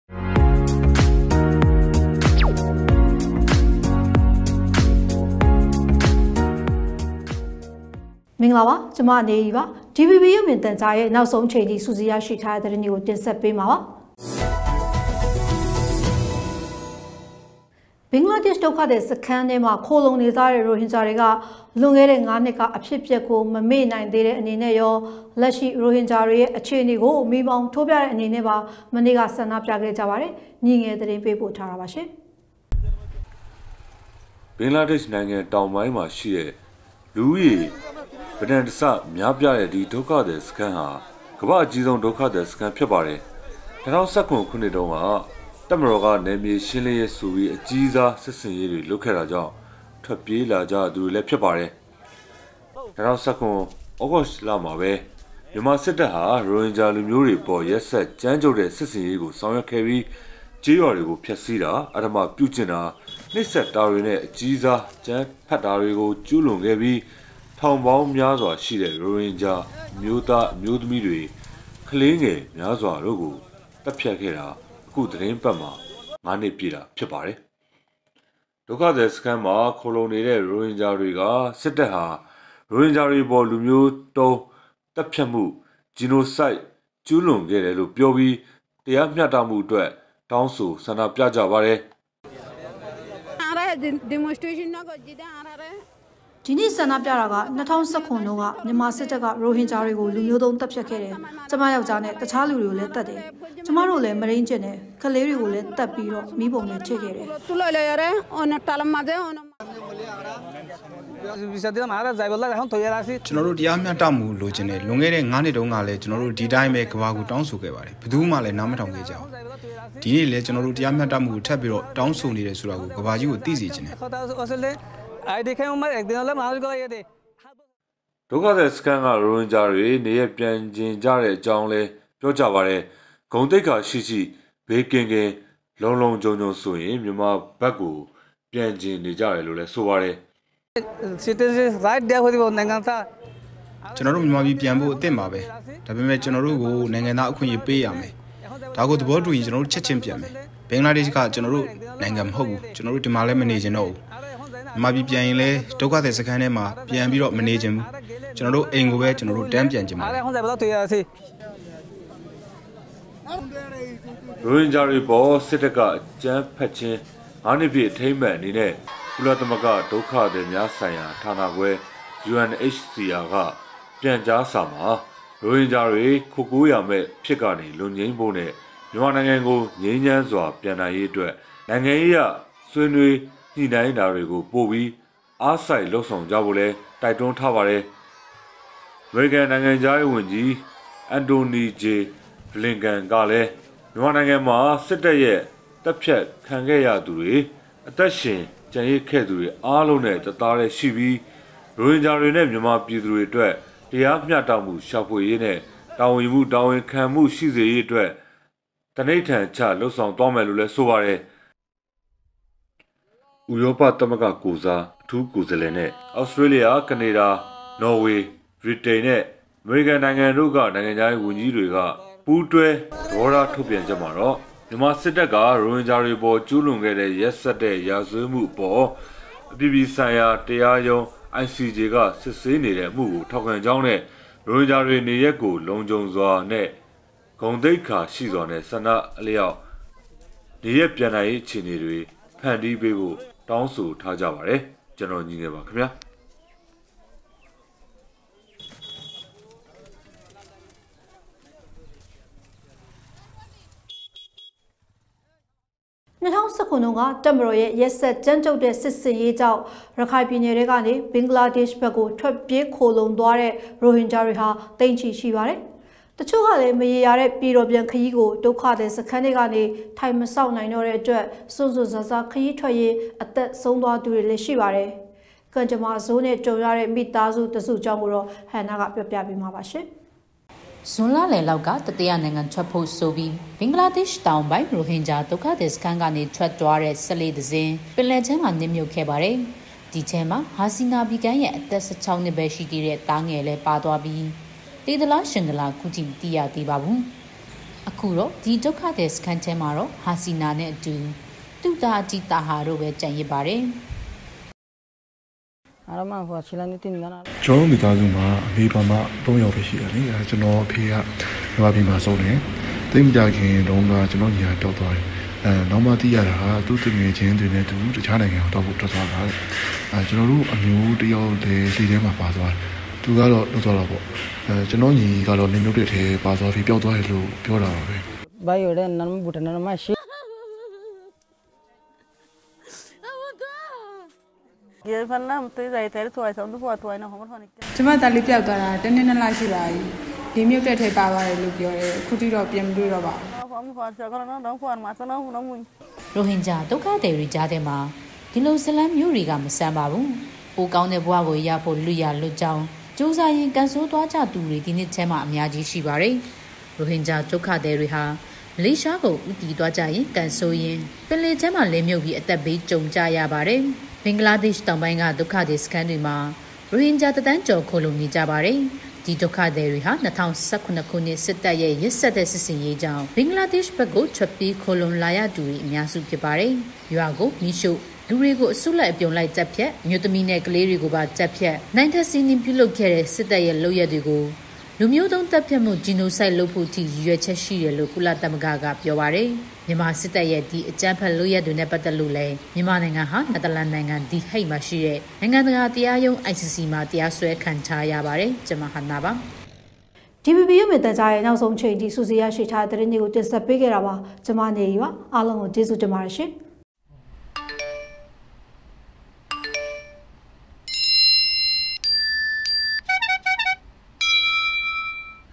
DVB Digital နေ့လယ် ၁ နာရီ သတင်း (၂၆ရက် ဩဂုတ် ၂၀၂၂)
- ဘင်္ဂလားဒေ့ရှ် ဒုက္ခသည်စခန်းထဲမှာ လက်ရှိ ရိုဟင်ဂျာတွေရဲ့ အခြေအနေကို မီးမောင်းထိုးပြတဲ့အနေနဲ့ ဆန္ဒပြ။
- ရခိုင်ပြည်နယ်ထဲကနေ ဘင်္ဂလားဒေ့ရှ်ဘက်က ထွက်ပြေးခိုလှုံသွားတဲ့ ရိုဟင်ဂျာ မိသားစုတစုရဲ့ ကံကြမ္မာ။